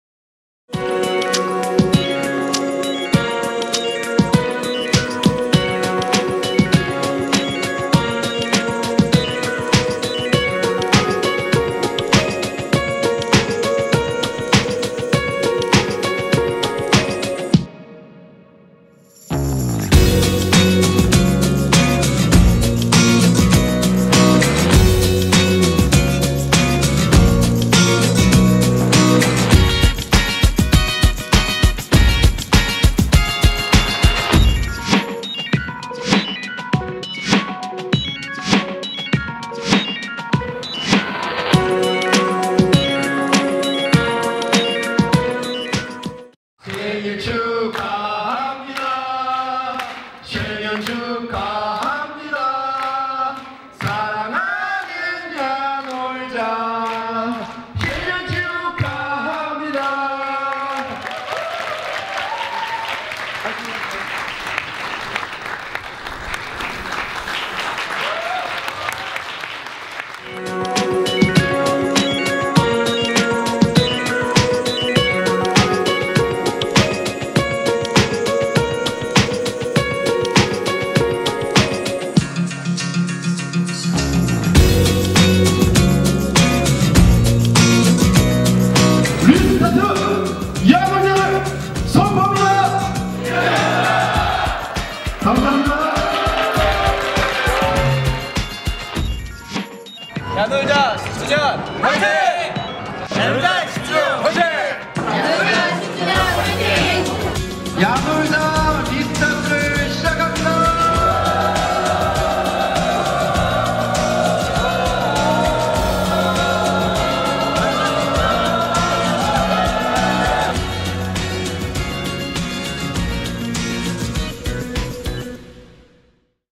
야놀자 10주년 행사영상
2015.3.2 논현동 임피리얼필리스호텔에서 진행 된 야놀자 10주년 기념 행사 "RESTART ; YANOLJA" 행사영상입니다.